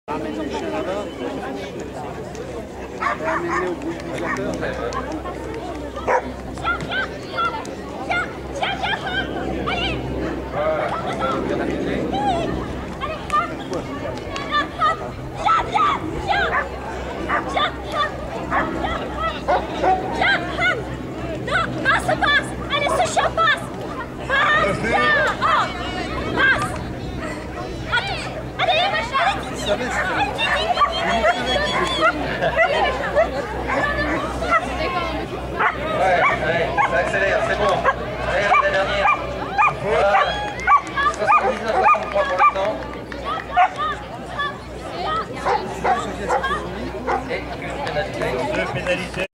AULET CHOCOLAT Barb manche 2 jumping cat C
Finale du Grand Prix de France d'agility de la SCC, Le Creusot, juillet 2010, manche 2 jumping, catégorie C